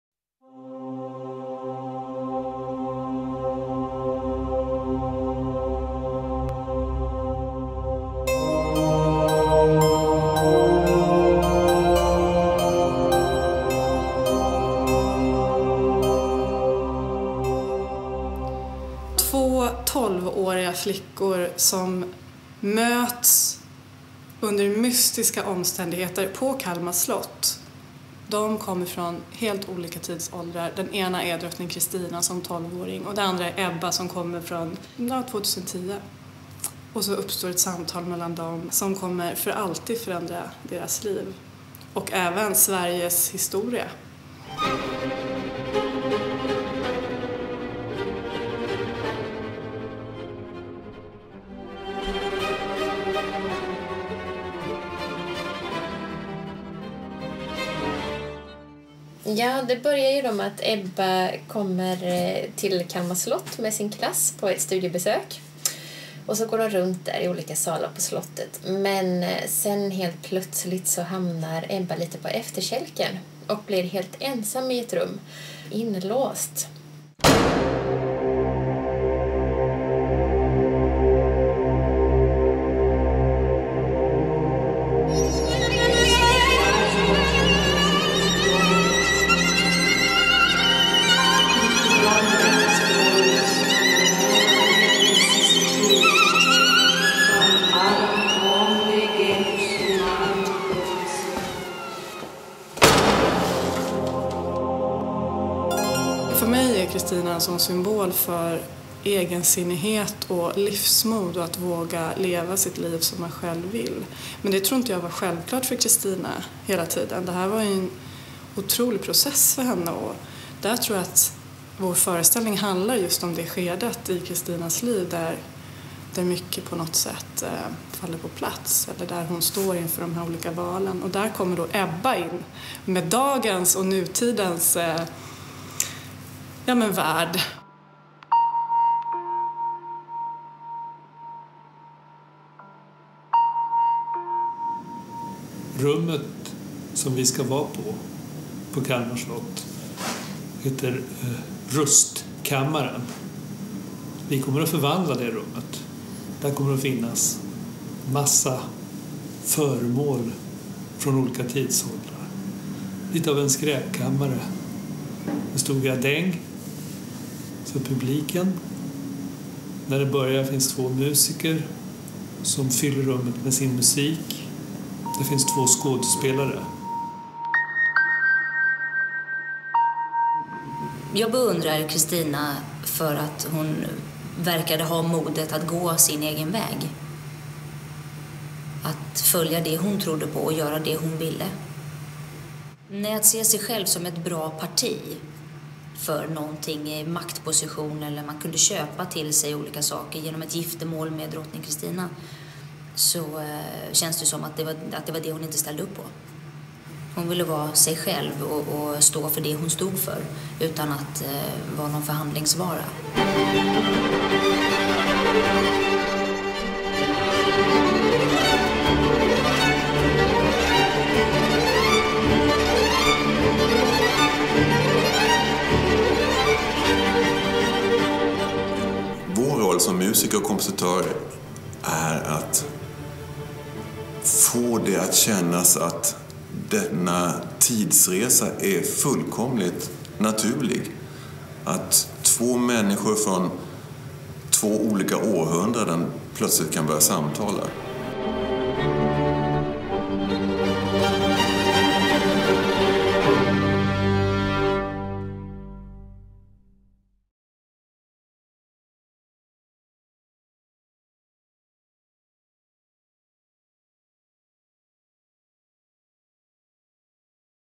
Kristinas flykt
En thrillerartad fantasyhistoria i slottsmiljö om livets stora frågor!
Nyskriven text och musik för Byteatern och Länsmusiken på Kalmar slott